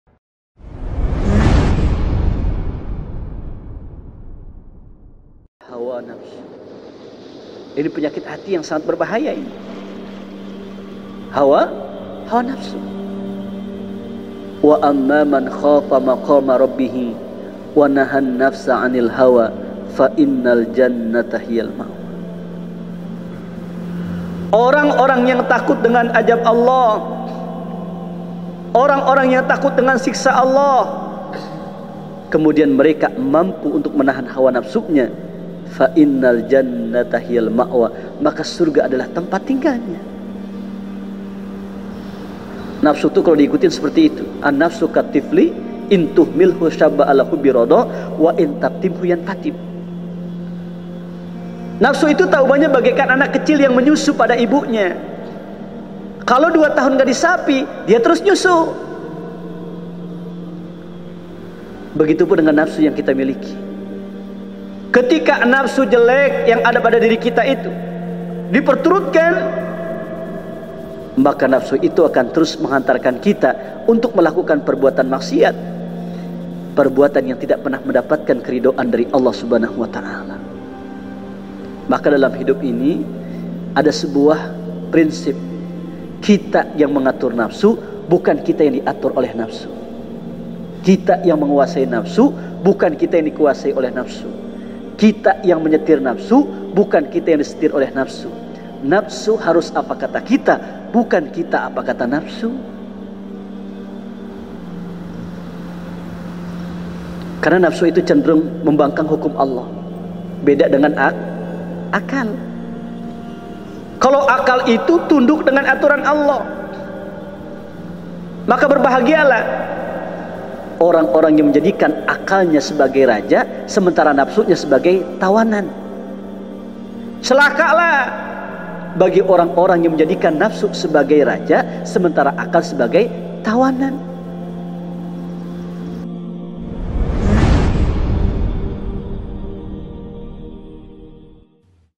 Akal Sebagai Raja, Nafsu Sebagai Tawanan - KH. Iwan Jawawi | Nasihat Halaqoh Shubuh
Official Media Masjid Az-Zikra Sentul, Bogor

Donasi Support Az-Zikra Media No. Rek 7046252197 a/n Yayasan Az-Zikra, Bank Mandiri Syari'ah